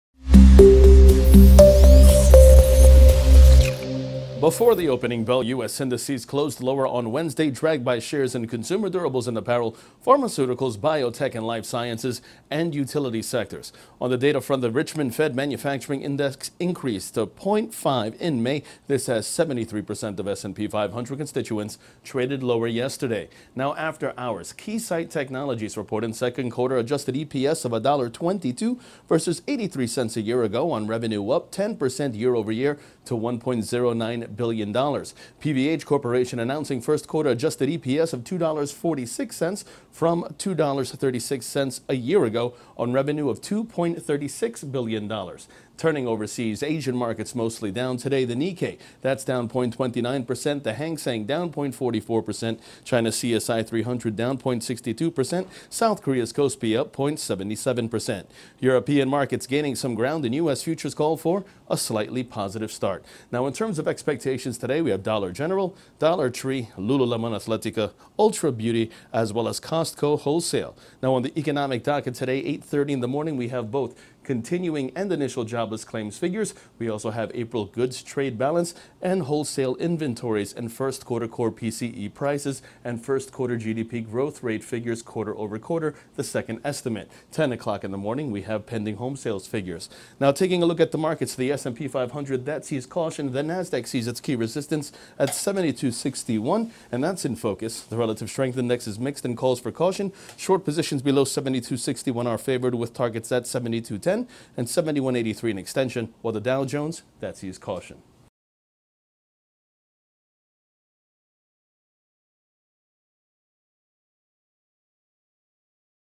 Stock futures rise, Asia slides overnight, Nasdaq in focus
Stocks are set to see a positive start Thursday as investors turn to more data and several key earnings reports for direction.. Also, Asia mostly slides overnight and the Nasdaq is in focus.